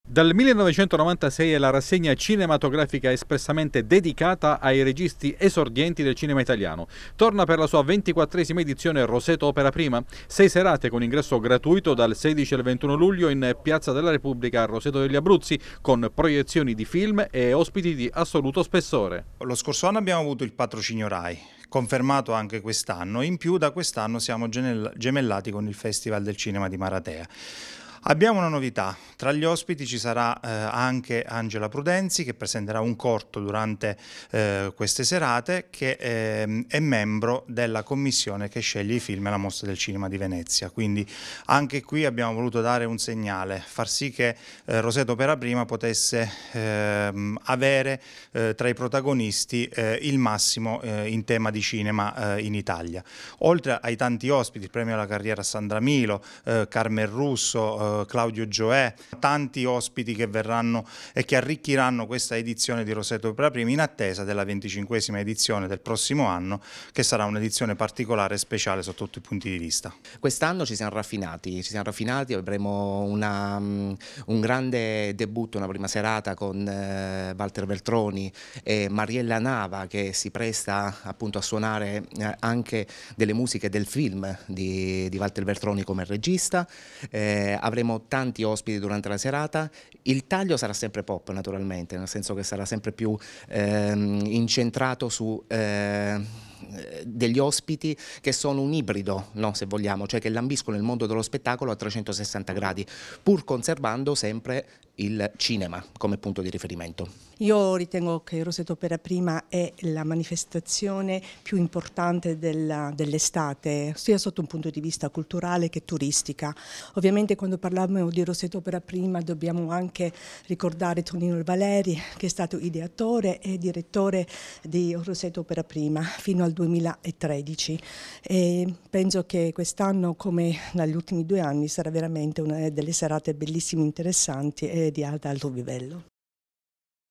Dal 16 al 21 luglio “Roseto Opera Prima 2019”
Walter Veltroni, Gigi Marzullo, Sandra Milo. Sono solo alcuni dei prestigiosi ospiti che prenderanno parte alla 24^ edizione di “Roseto Opera Prima” in programma Roseto degli Abruzzi dal 16 al 21 luglio